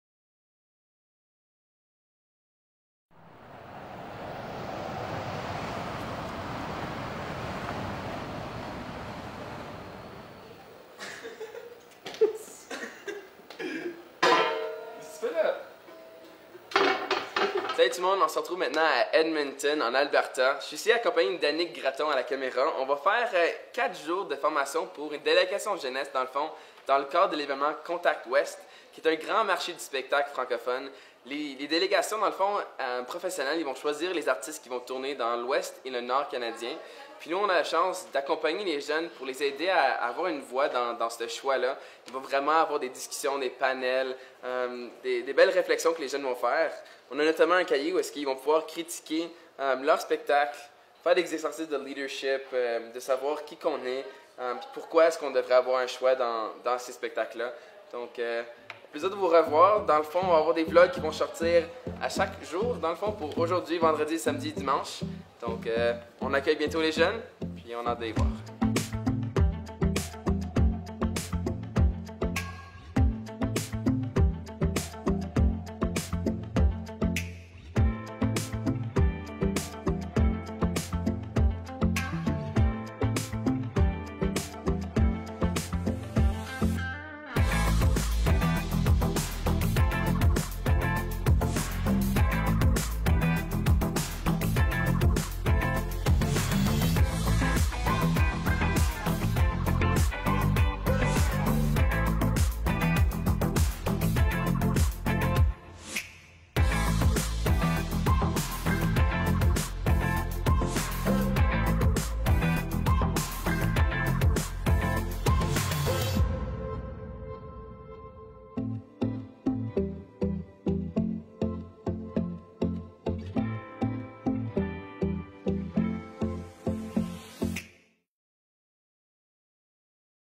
La première journée à Contact Ouest
Voici le vlog de la première journée à Contact Ouest! La délégation jeunesse est en pleine découverte artistique 🤩

On y voit l’Honorable Mme Leela Aheer, ministre de la culture, du multiculturalisme, de la condition féminine et responsable du Secrétariat francophone, ainsi que Martin Theberge, président de la Fédération culturelle canadienne-francaise.

Merci au Francophonie Jeunesse de l'Alberta, RAFA et au RGE ! Chapeau à notre magicien Danick Gratton pour la réalisation de cette capsule 😉